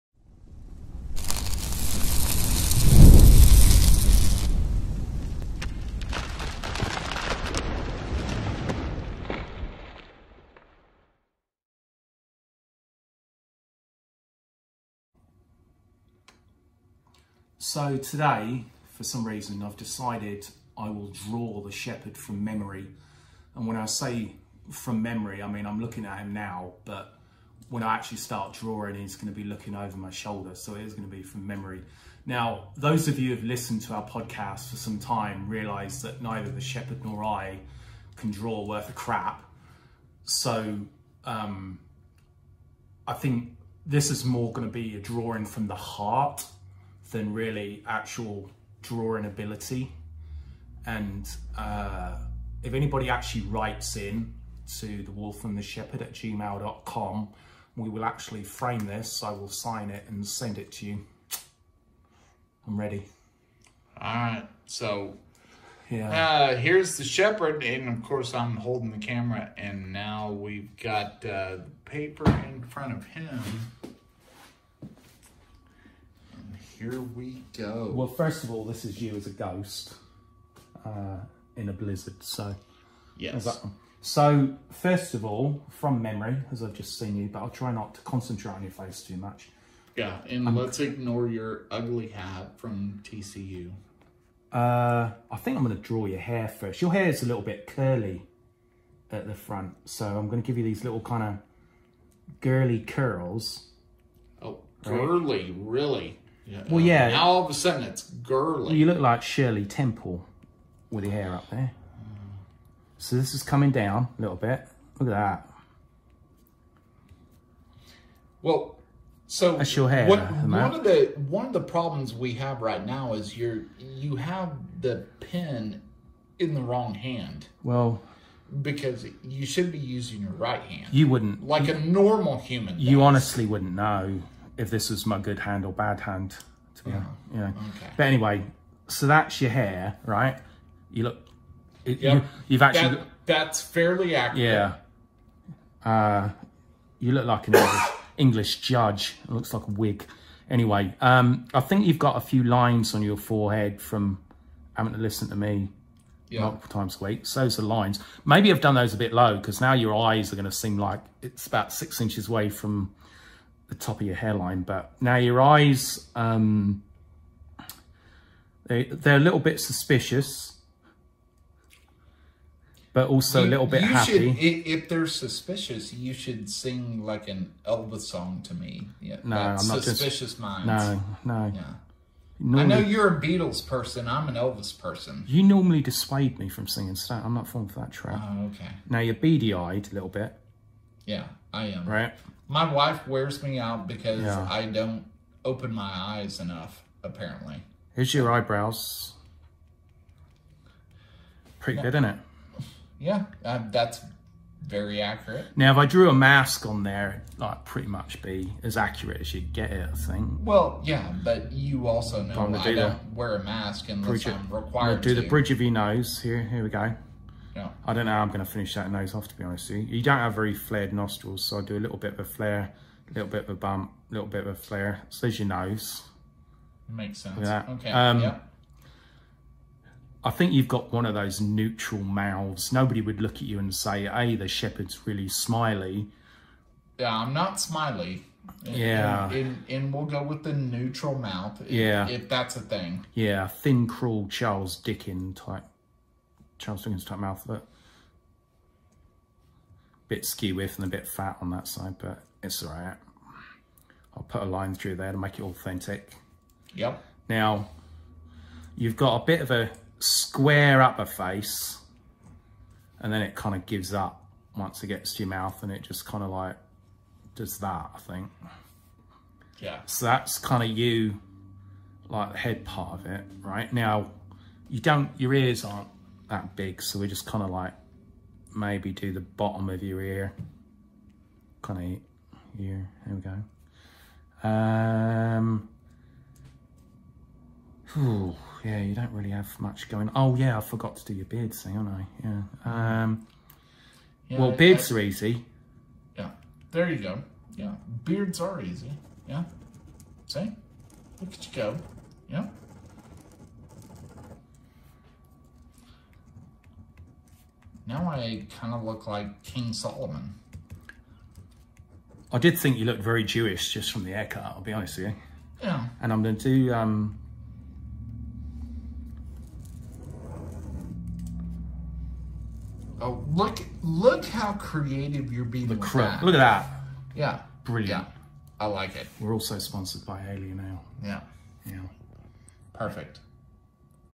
The Wolf DRAWS The Shepherd
The Wolf channels his inner Pre-K drawing skills to draw the Shepherd from memory; that memory being from 5 seconds previous to starting.